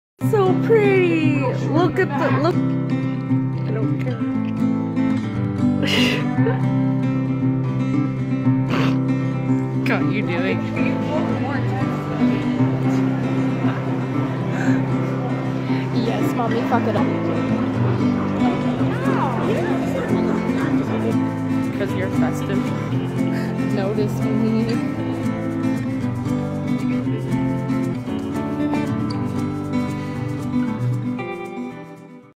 8/12/17
a jolly good time adventuring around the mall with an og pretzel n this tall little fagatron. it was fun, afterwards my mom and i shared a gourmet celebrator mcdonalds meal for my great concert the night before. glad i got to vlog this. psa: santa photos were aqcuired and theyre iconic.